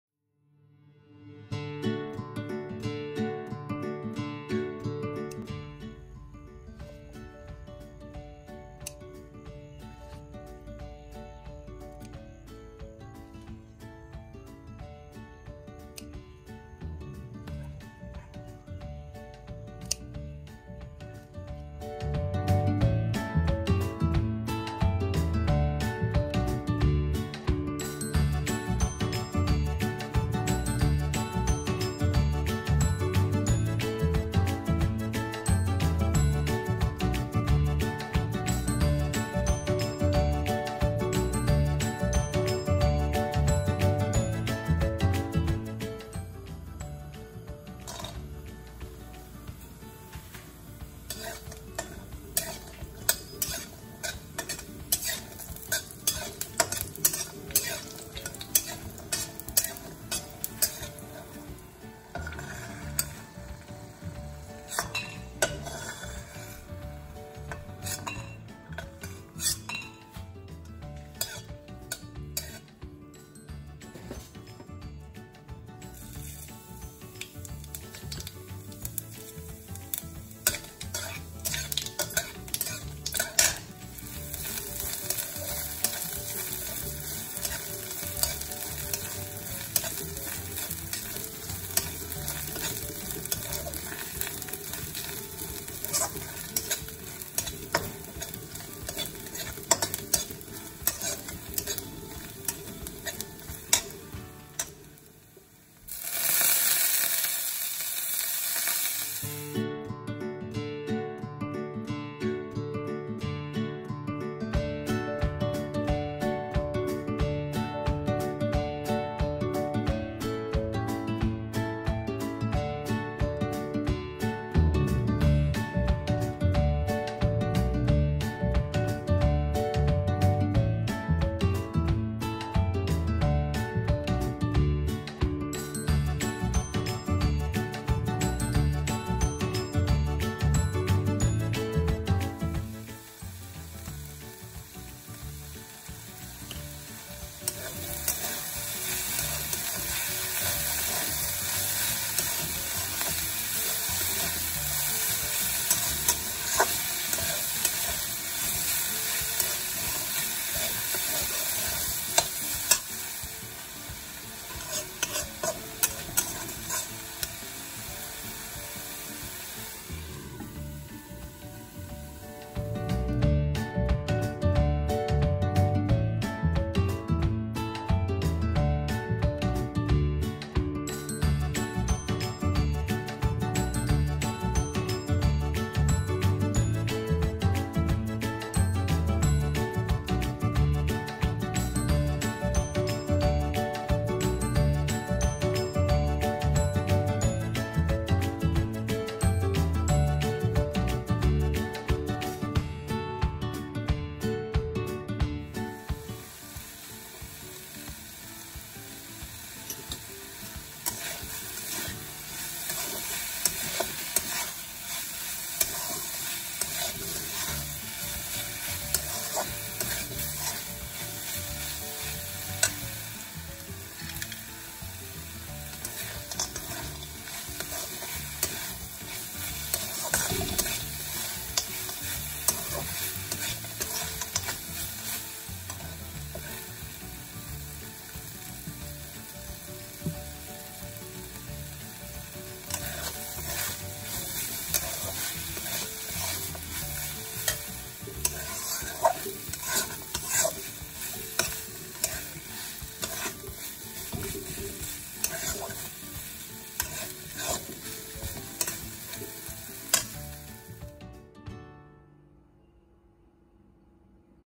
No Onion Garlic Red Spinach Recipe/Lal Saag Bhaji/Red Amaranth/লাল শাক ভাজি/Satvik Food Recipes
Lal saag is a good source of iron, vitamin C and A and other essential minerals. quick and easy to make.

No Onion Garlic Recipe # Easy And Simple Recipe # Healthy Saag Recipe # Pure Vegetarian Food Recipe # Satvik Food Recipes